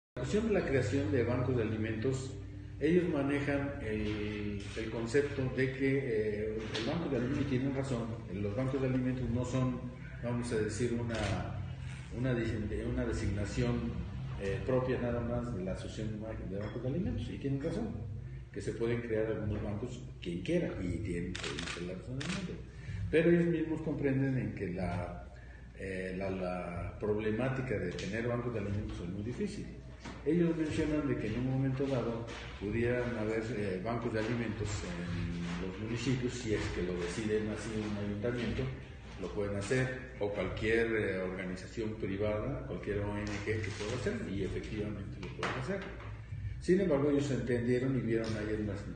Continúa Edomex implementando Bancos de Alimentos
TOLUCA, Edomex., 19 de febrero de 2019.- La creación de nuevos bancos de alimentos es un proceso complejo, por lo que se podría aprovechar el ya existente en el Estado de México y así lograr llegar a más personas con carencia alimentaria, señaló Alberto Canul Juárez, director del Banco de Alimentos de la entidad IAP., esto tras la iniciativa de ley que impulsó el partido Verde Ecologista de México.

Tras aprobarse en comisiones la creación de Bancos de Alimentos y generar incentivos fiscales para los donadores, Canul Juárez se reunió con el diputado José Couttolenc Buentello, donde dijo, se logró aclarar que sí se pueden crear bancos, pero será decisión de cada municipio u organización.

Asimismo, el director del Banco señaló que durante la visita a sus instalaciones, el diputado se dio cuenta de todas las implicaciones que conlleva el manejo de un banco, desde instalaciones, logística, personal, manejo de alimentos y más; asimismo, indicó que se aclaró que la iniciativa va en función de generar que no se desperdicien los alimentos y se puedan llevar a personas que lo necesiten.

El director señaló que buscan tener una cobertura más amplia en la entidad, ya que únicamente cubren a 135 mil personas, en 45 municipios, lo que representa el 6 por ciento de las personas con carencias y quieren llegar al oriente y sur de la entidad; y con los estímulos que determina la iniciativa podría incrementar el número de donadores para lograrlo, “nosotros podemos ser el vínculo para hacer llegar el alimento de las familias”, finalizó.